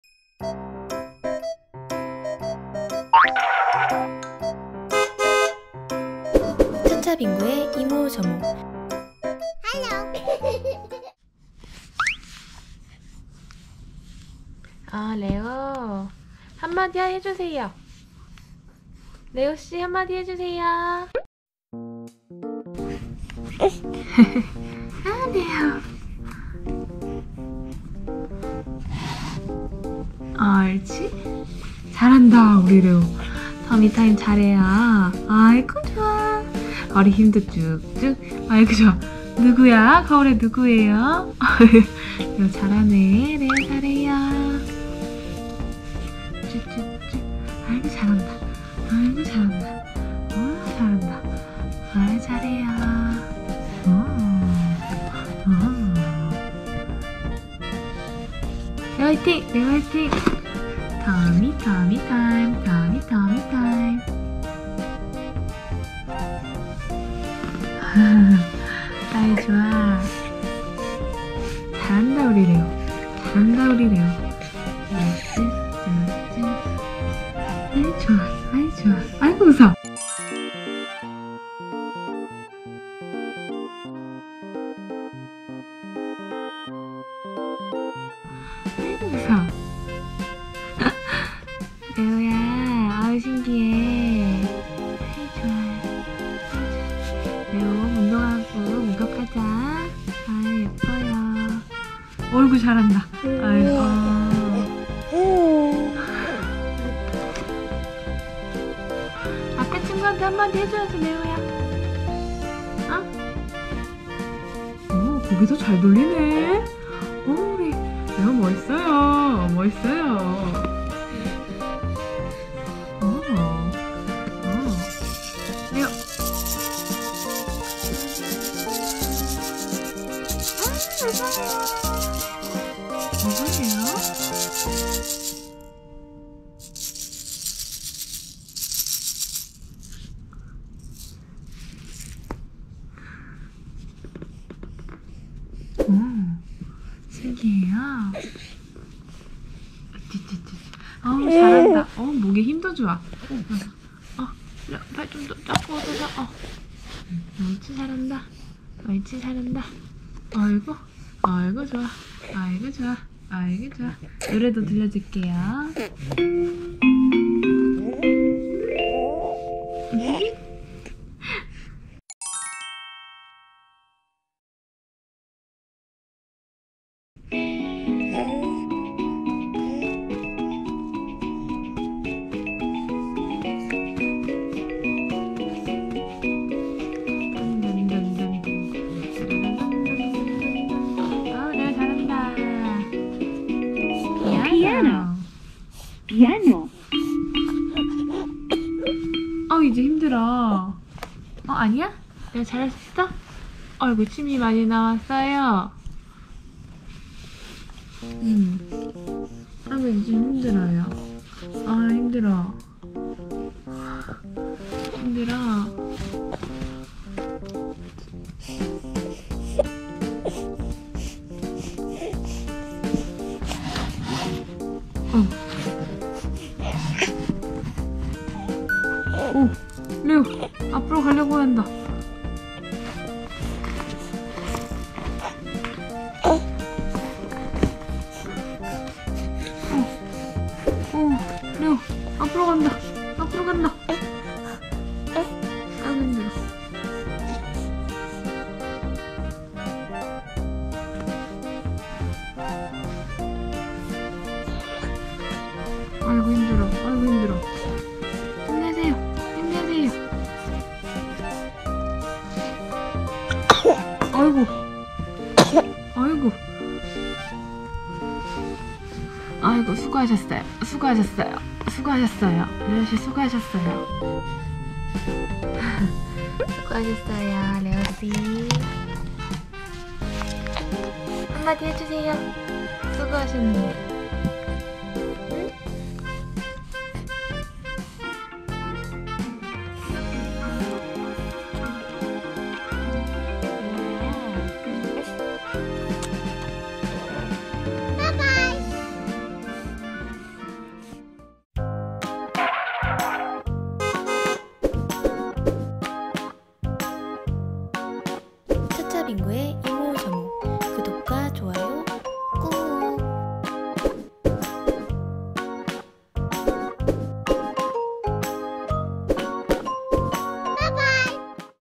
[3개월아기] 거울에 비친 자기 얼굴보고 좋아하는 아기 | 미국 육아 | 미시간 생활
12주차때 찍었던 영상이에요!
지금은 터미타임을 더 잘 한답니다. (곧 100일입니다 :D)
헤헤.. 터미타임은 아기의 상체 힘을 길러주기 위해 엎어(엎드려) 놓는 자세를 말하는데 신생아때부터 해도 좋다고 해요.
그래서 레오아빠는..신생아때부터 꾸준히 시켰답니다ㅋㅋ